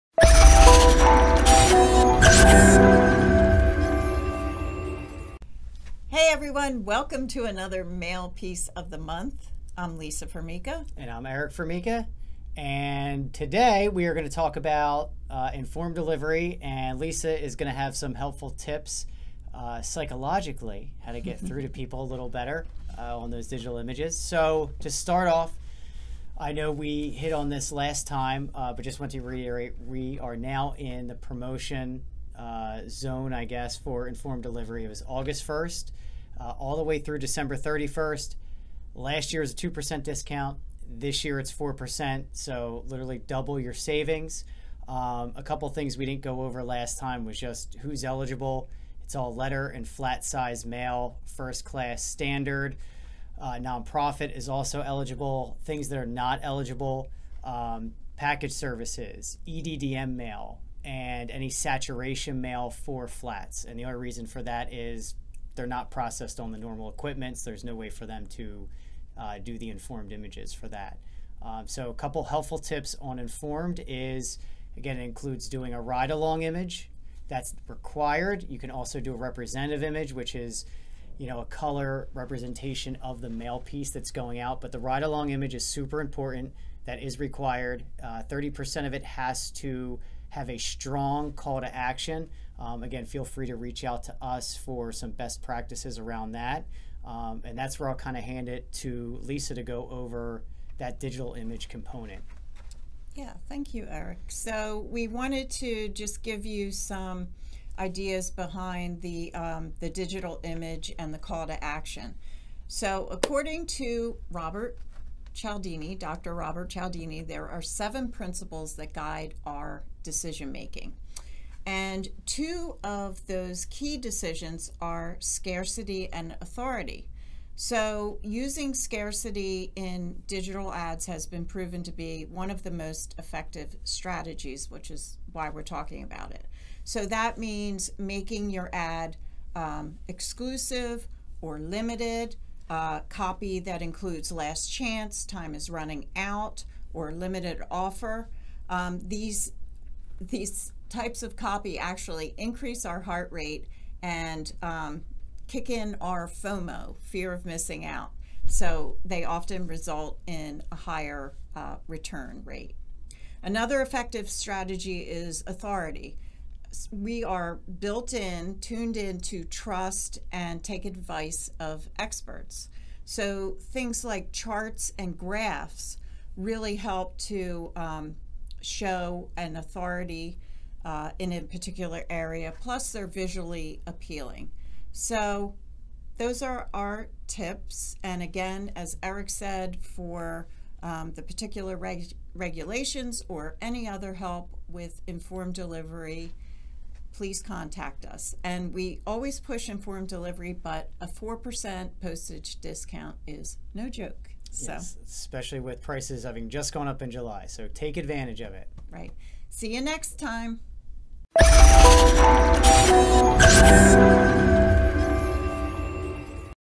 August 2022: Informed Delivery
Save 4% on postage during the USPS Informed Delivery Promotion! The sale runs from August 1 through December 31, 2022.

Not only can you save postage, the digital ad on the USPS Informed Delivery platform gives you another opportunity for exposure, and has been shown to help increase response rates.

Check out this month's Mail Piece of the Month video for details.

fmi direct mail inc.